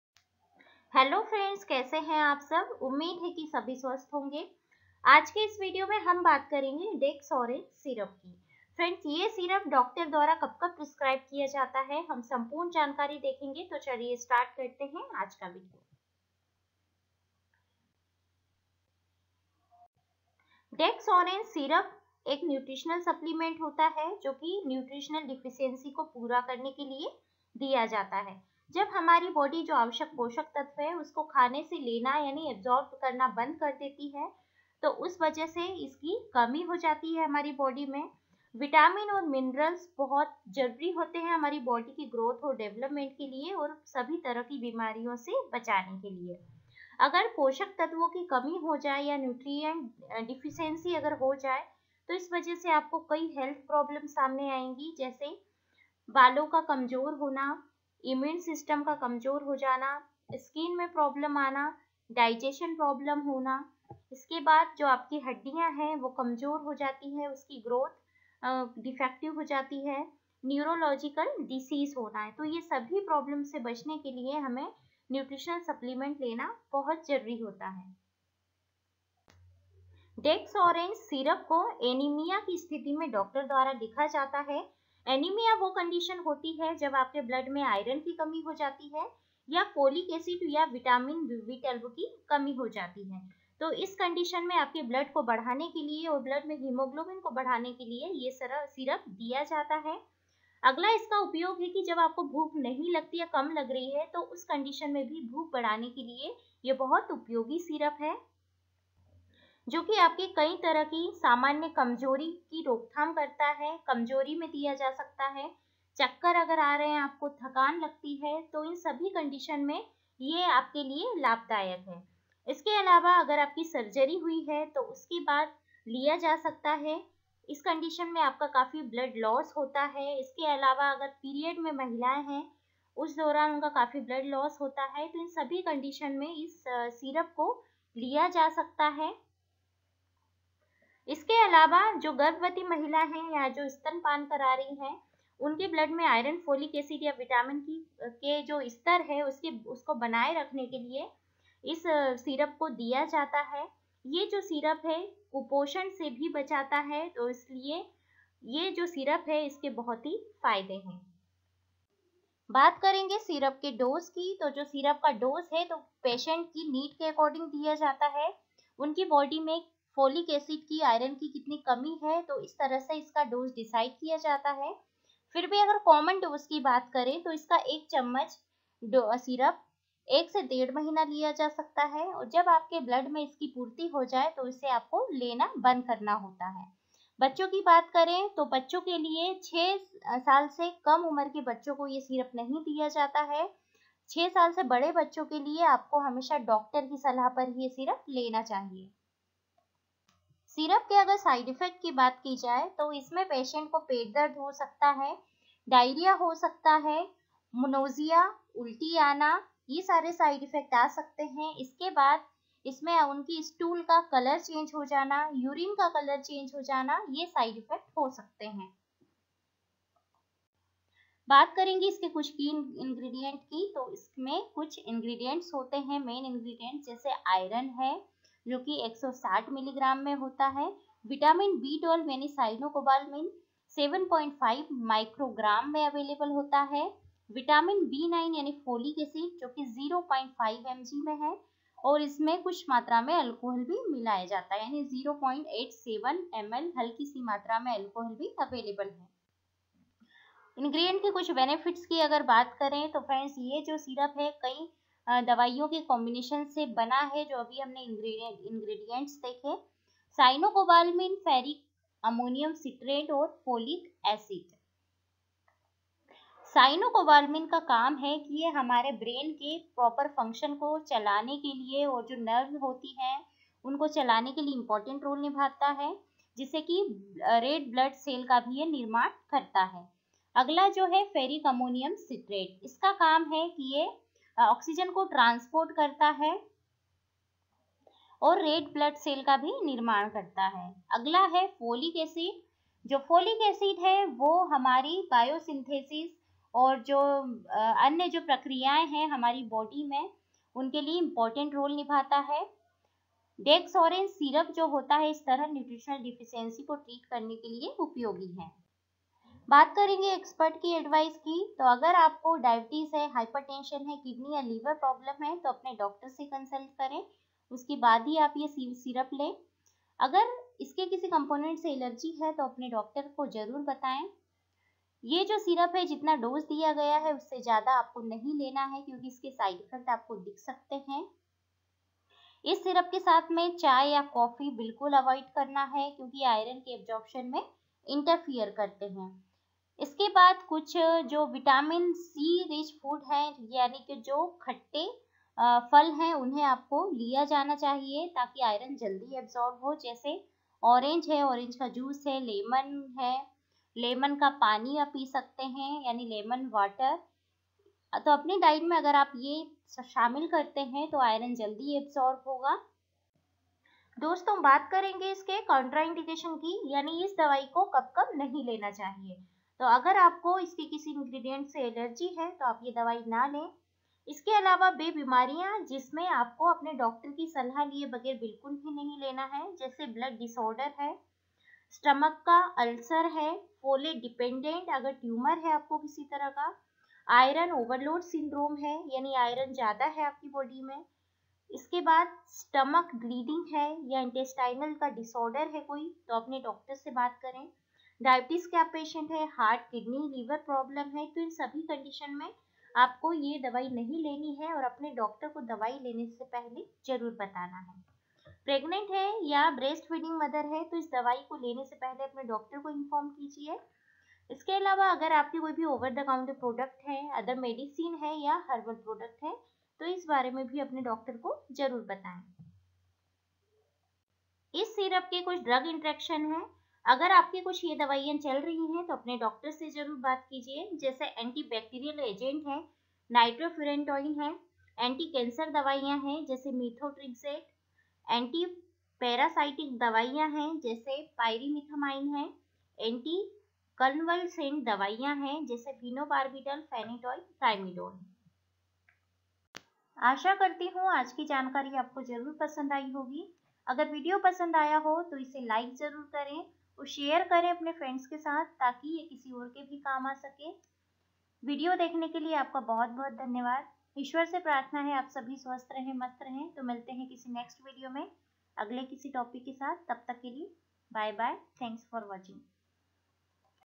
DEXOREANGE SYRUP KE FAYDE | Hematinic Syrup
Dexorange Syrup 200 ml belongs to the group of medicines called nutritional supplements used to treat and prevent nutritional deficiencies such as iron deficiency, anaemia, and folic acid deficiency. A nutritional deficiency occurs when the body does not absorb or get enough nutrients from food. Vitamins and minerals are necessary for body development and the prevention of diseases.

This video contain information about dexorange syrup use in hindi including
dexorange syrup benefits and use
for nutritional benefits weight gain
syrup dose
composition
side effects
drug interaction
contraindication
expert advice

Medicinal Benefits:-
Dexorange Syrup 200 ml is a combination of three supplements, namely: Cyanocobalamin, Ferric ammonium citrate, and Folic acid

. Dexorange Syrup 200 ml is used to treat and prevent nutritional deficiencies.
Cyanocobalamin helps in the proper functioning of the brain, nerves and the production of red blood cells.
 Ferric ammonium citrate acts as a source of iron which helps in the transportation of oxygen throughout the body and the production of red blood cells.
Folic acid is necessary for various bodily functions, such as nucleotide biosynthesis and remethylation of homocysteine.
 Together, Dexorange Syrup 200 ml helps in treating nutritional deficiencies.

Side Effects of Dexorange Syrup 200 ml
Stomach upset
Constipation
Diarrhoea
Nausea
Vomiting

Drug-Disease Interactions: Inform your doctor if you have a blood disorder or stomach ulcer.